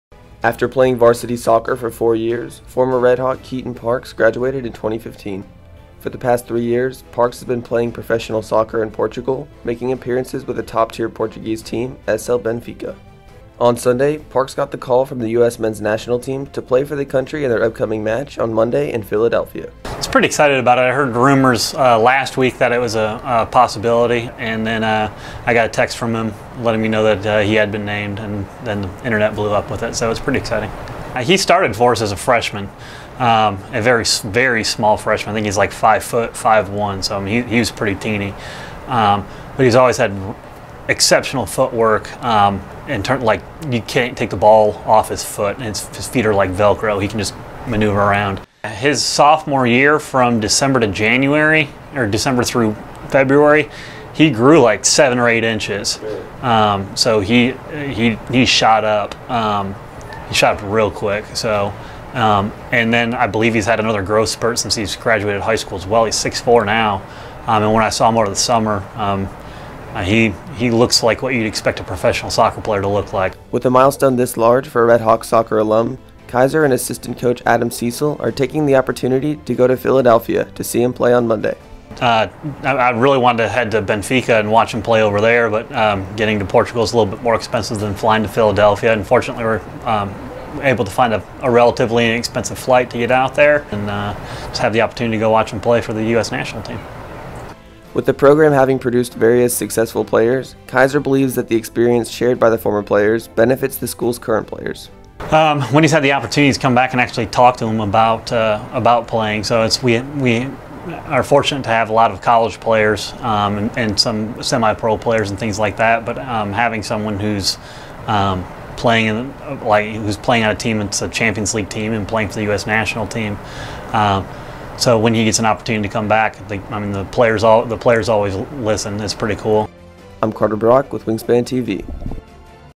Keaton Parks
Alum Keaton Parks is making his debut on the U.S. Mens' National Soccer Team.